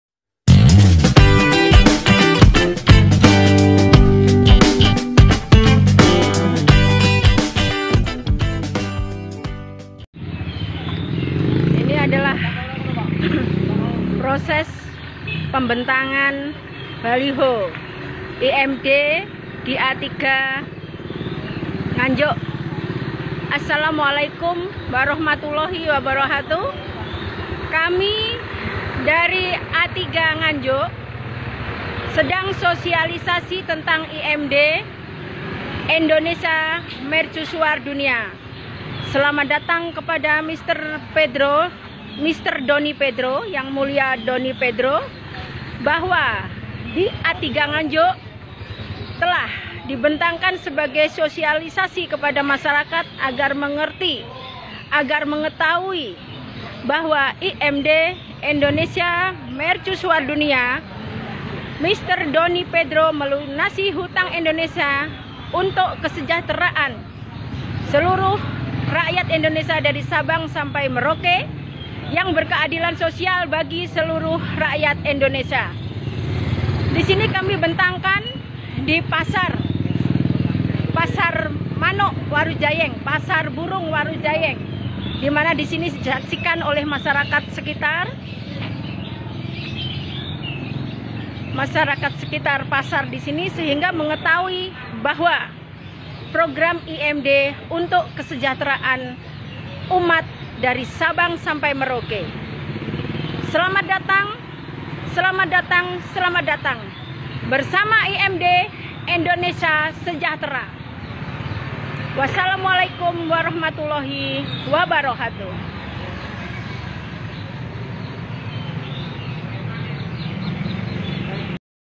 Ada yang mau melunasi hutang negara bro..!! Berita viral terbaru
Kini di indonesia muncul berbagai fenomena unik dan menggelitik. Stelah muncul berbagai kerajaan baru. Sekarang ada lagi nih. Pendekar baru yang akan memimpin dunia upin ipin